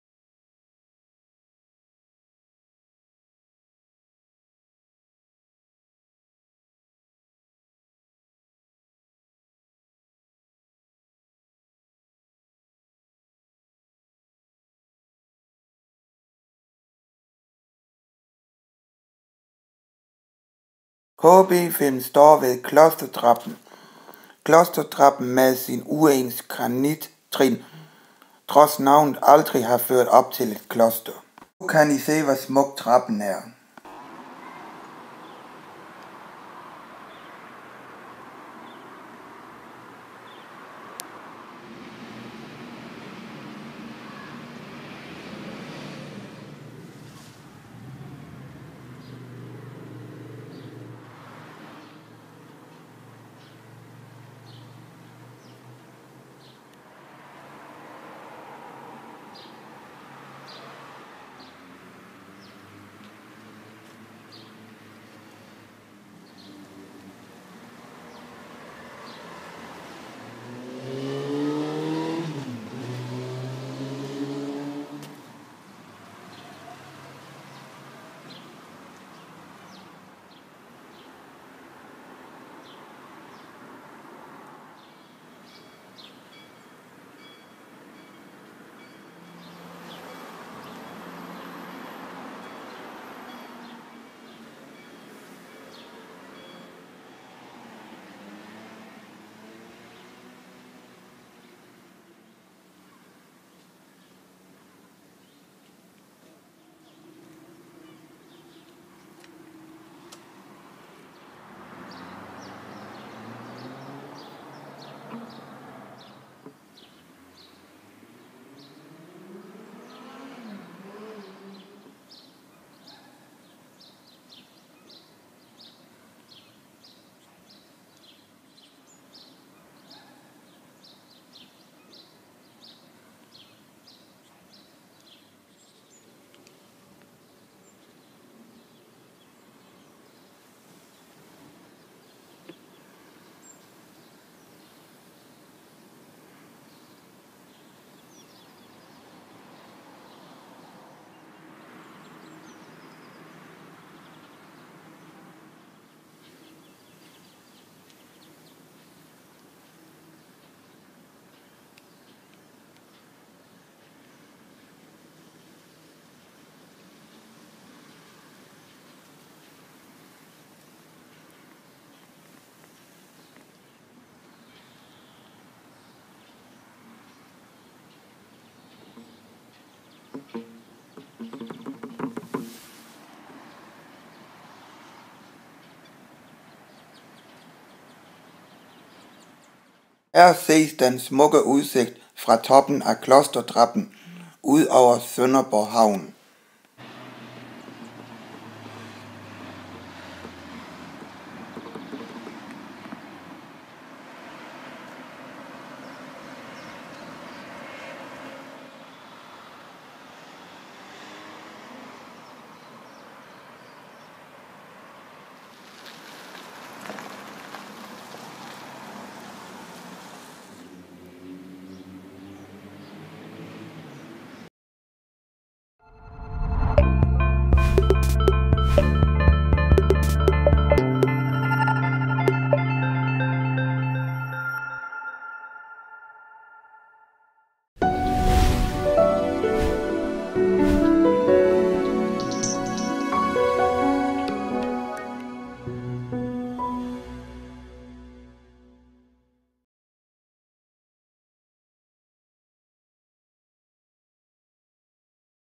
Klostertrappen i Sønderborg (HD 1080P)
her kan i se den smukke gamle klostertrappen der før op til jumfrustigen,